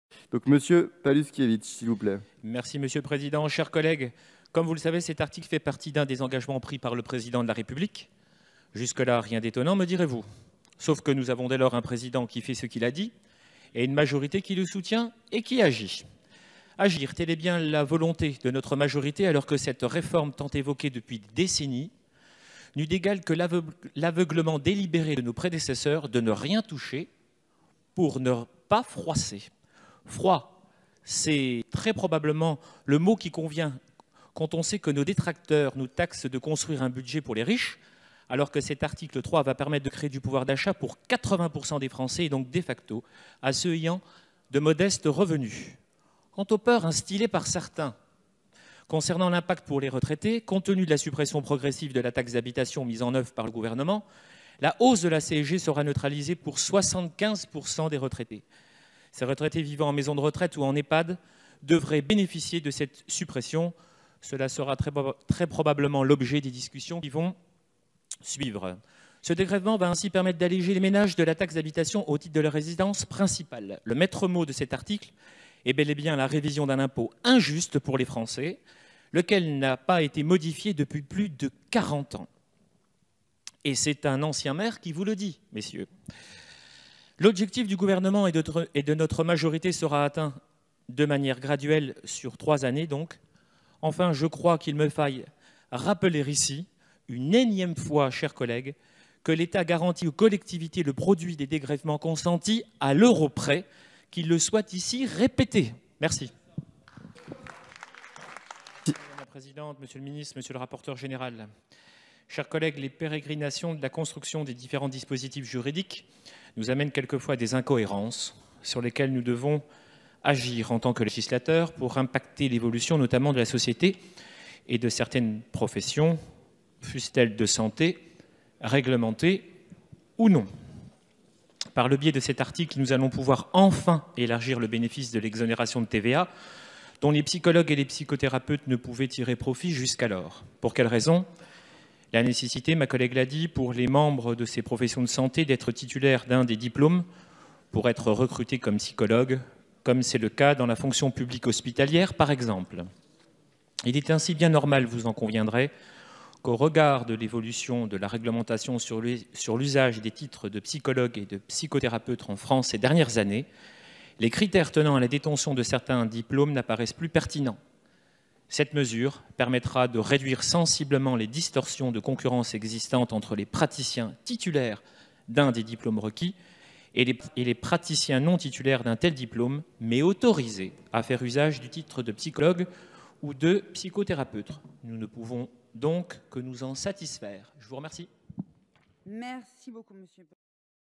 Mon intervention lors du Projet de loi de finances pour 2018 1ère partie (21.10.207)
Vis-à-vis de :
- l'Article 3 relatif au Dégrèvement de la taxe d'habitation sur la résidence principale,
- l'Article 6 concernant l'Extension de l'exonération de TVA applicable aux psychothérapeutes et psychologues.